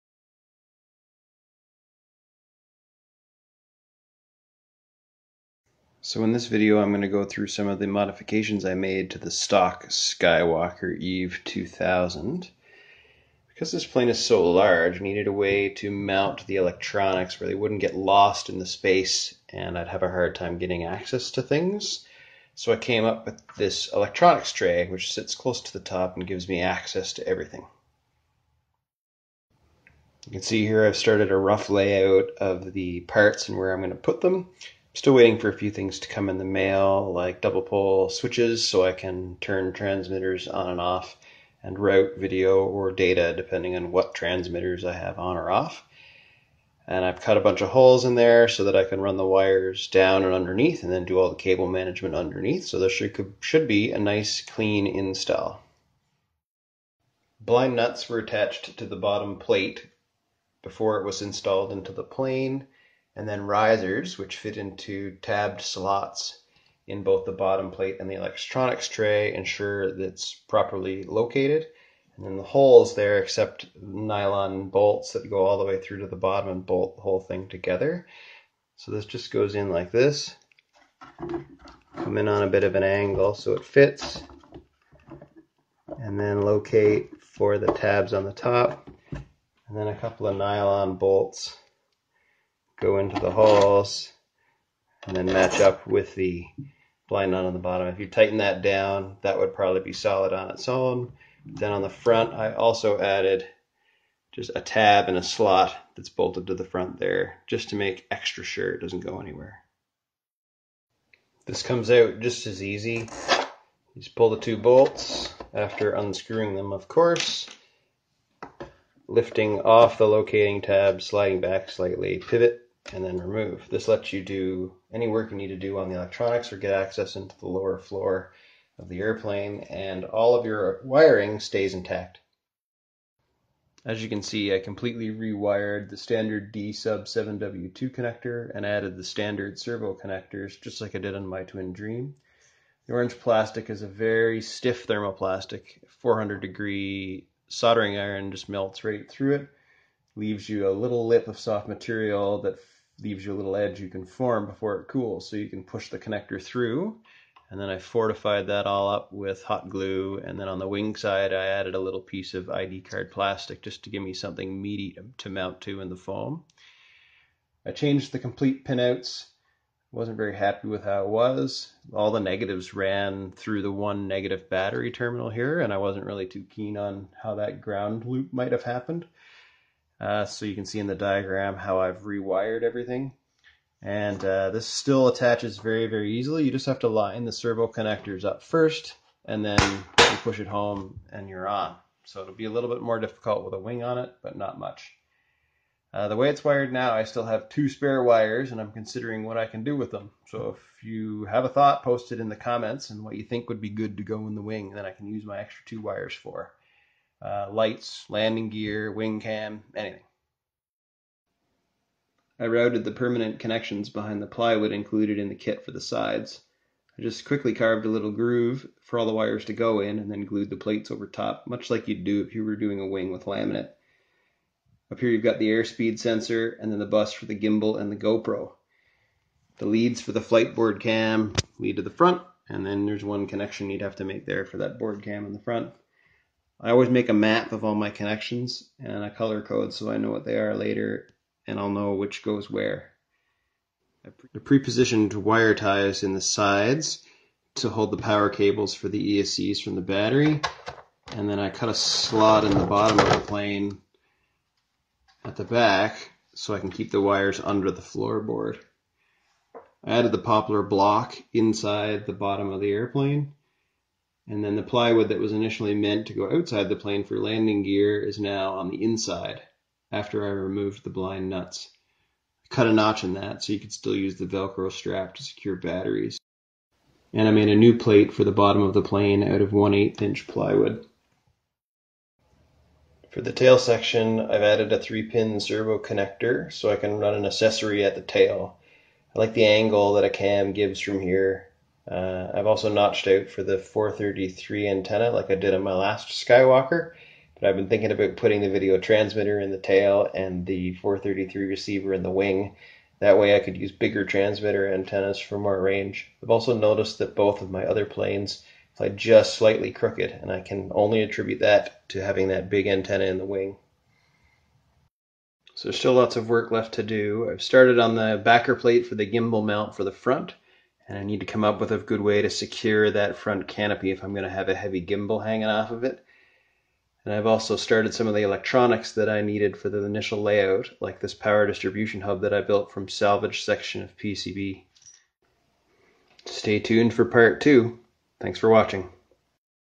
Skywalker eve 2000 build pt 1- fuselage custom interior
Heavy on customization, this build of the skywalker eve 2000 is going to take a while, but it'll be worth it!

Careful planning, modular construction and this time even paint!

In this video I go over the removable electronics tray added to the aft compartment, mods to the wing connectors, the fixed structural wiring, mods to the bottom of the plane and custom pitot tube holder. You'll see parts of the wiring diagram and how I preserved the massive internal space of the plane for payloads.